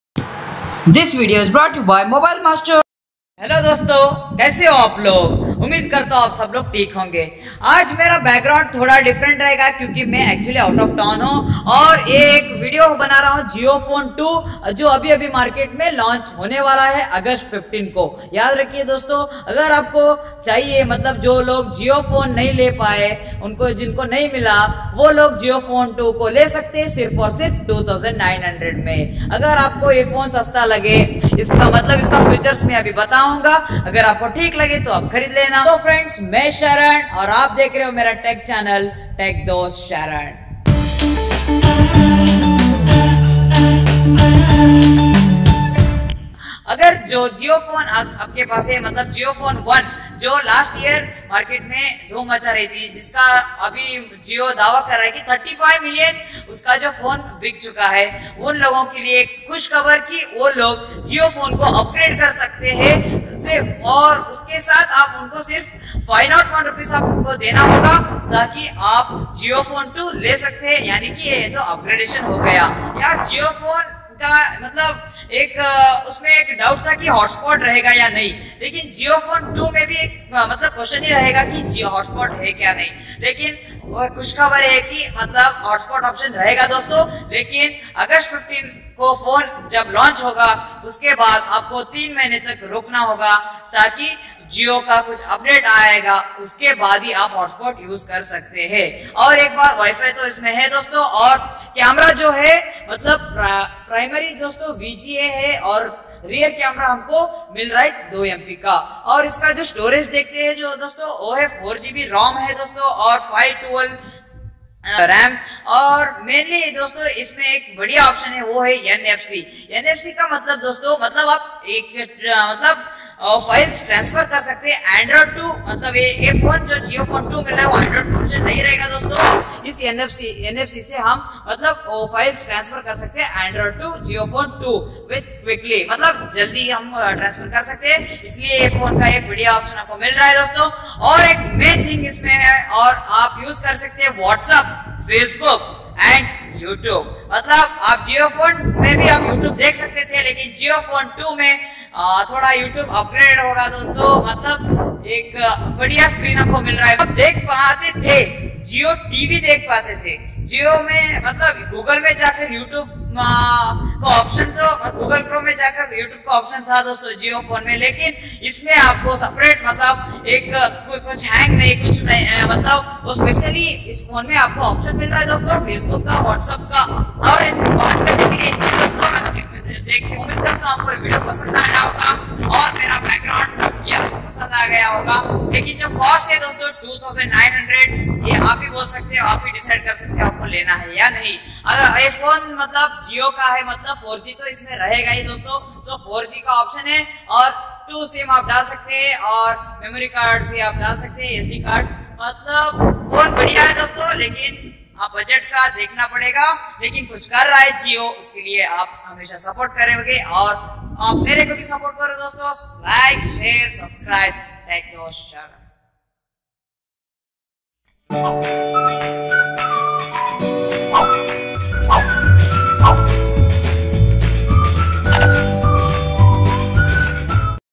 Jio phone 2 review and updates .does it have hotspot option ?? WHATS NFC
Sponsered By : "MOBILE MASTER", Super Market, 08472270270/273273.
Edited By : SHARAN PATIL, Gulbarga
Video Shoot By : SURESH RAJPUT, Gulbarga.